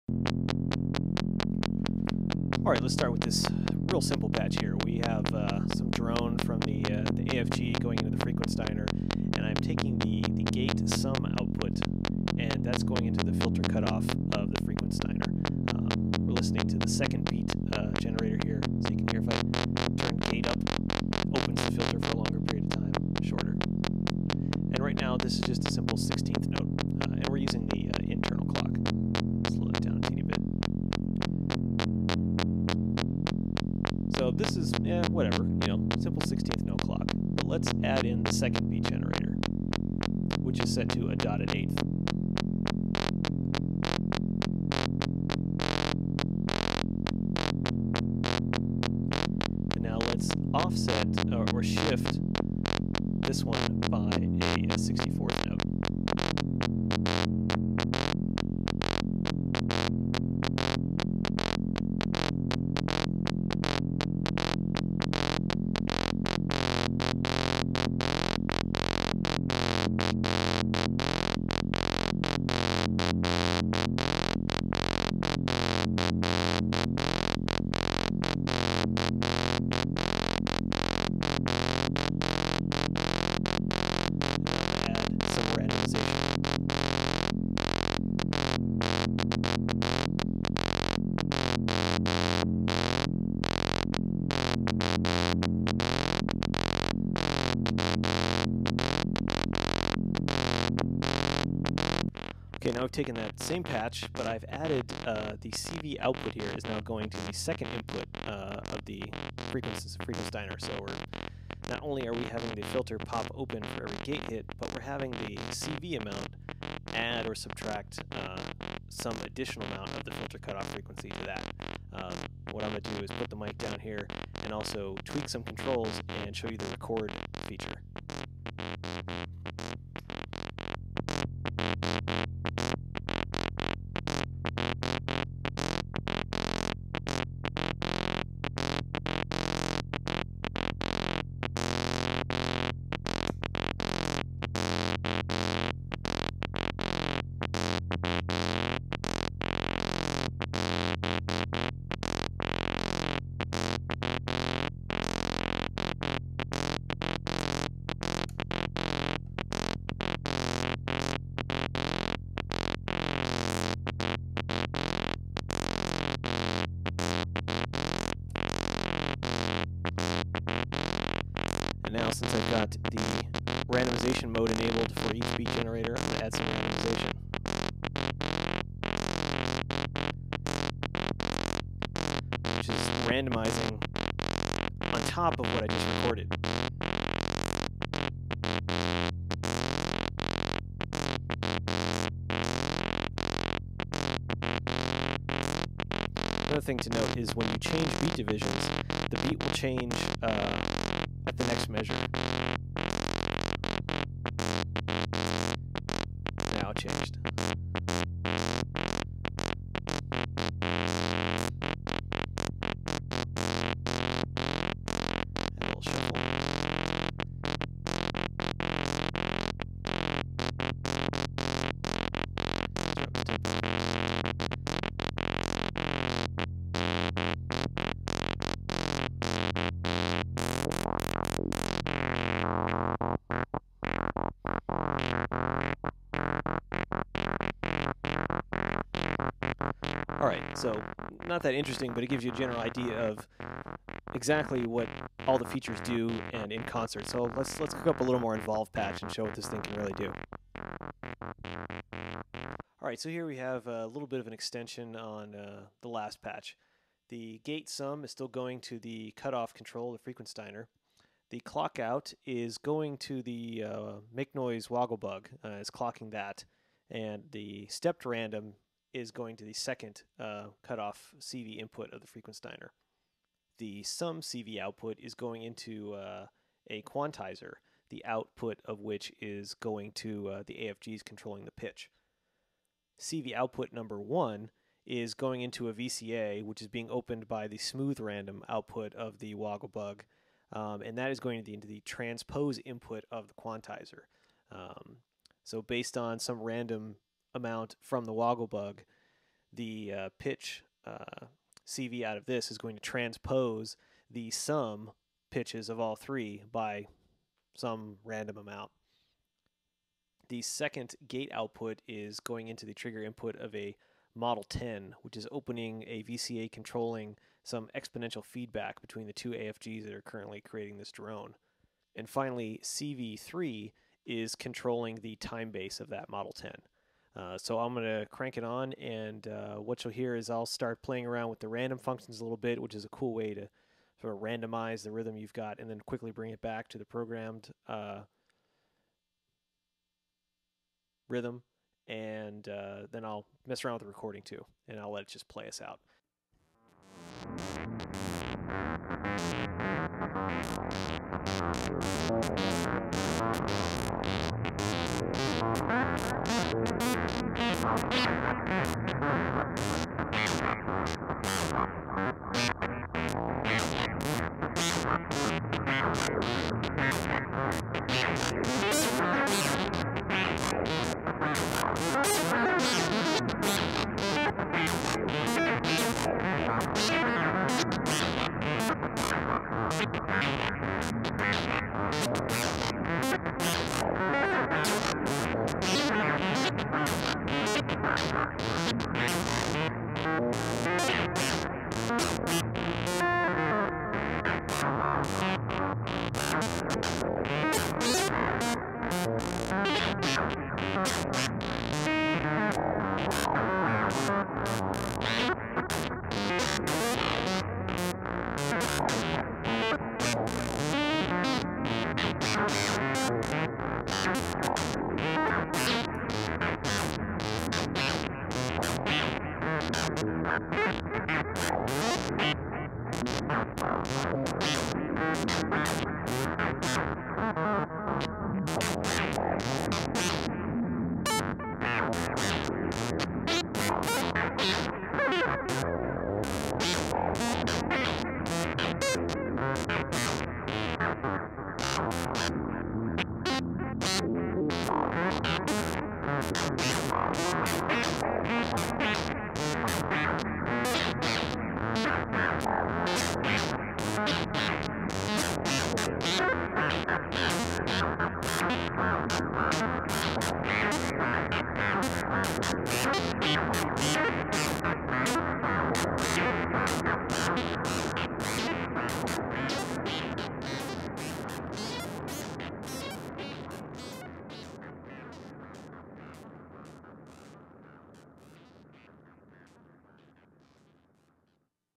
Flame Clockwork Demo Pt. 2
Example patches showcasing the various features of the Flame Clockwork eurorack module